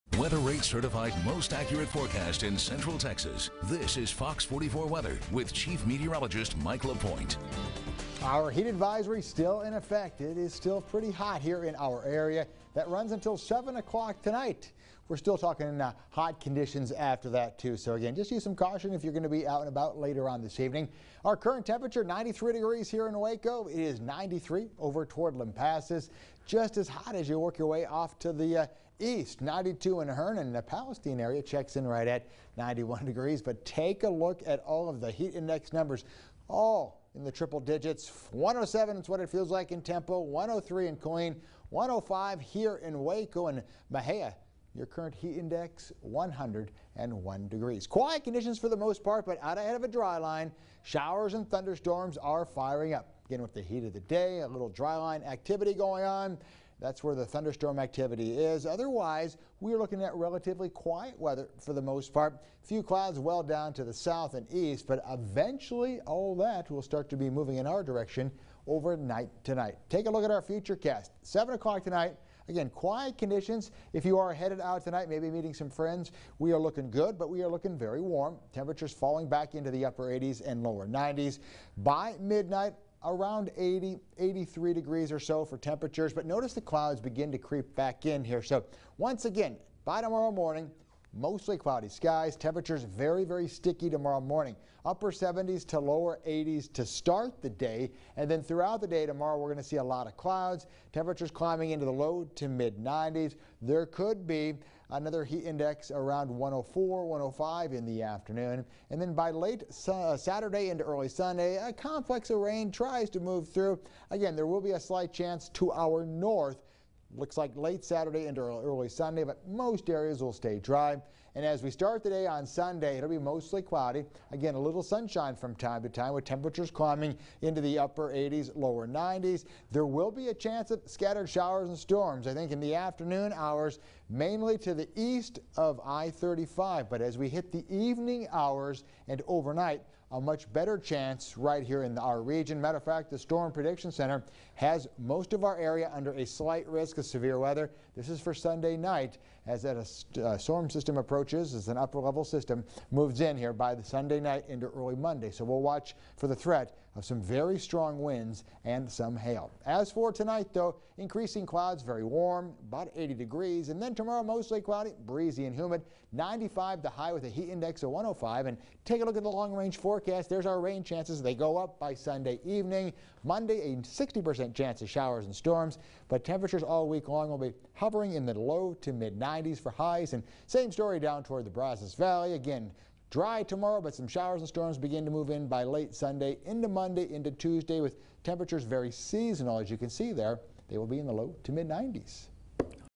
Friday 6:15 Forecast with Chief Meteorologist Mike LaPoint
The long-range forecast shows a chance of showers and thunderstorms just about every day. Chief Meteorologist Mike LaPoint has the latest in the video above.